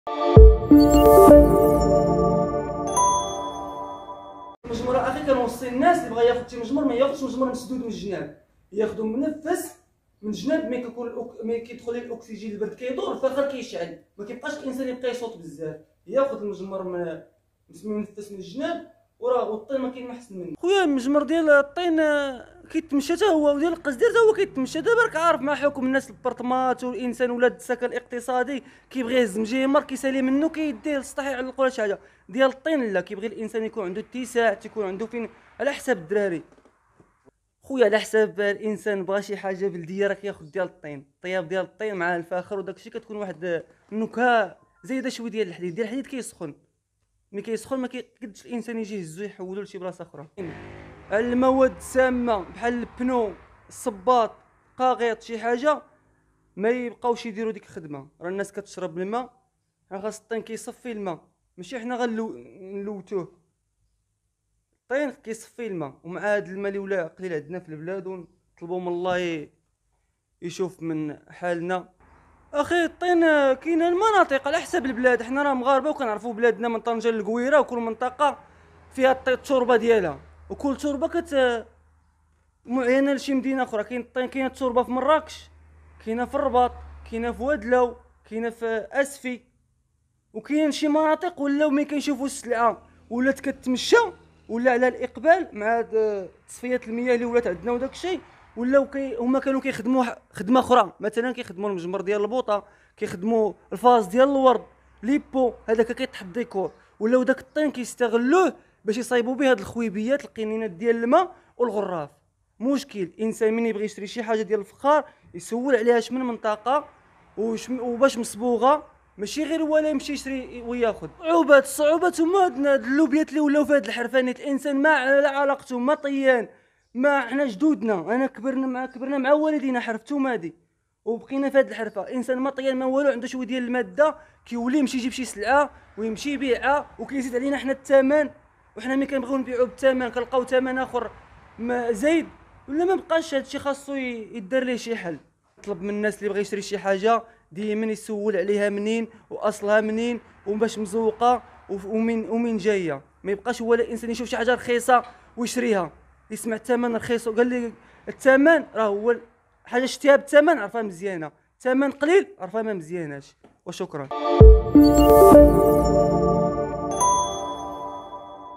نصائح صانع تقليدي لاقتناء "المجمر" والأواني الطينية
ونحن على أبواب عيد الأضحى، يرصد الفيديو تقديم صانع تقليدي مجموعة من النصائح لاقتناء أشهر الأواني الطينية لدى المغاربة وهي "المجمر" وأنواع أخرى من الأواني الطينية.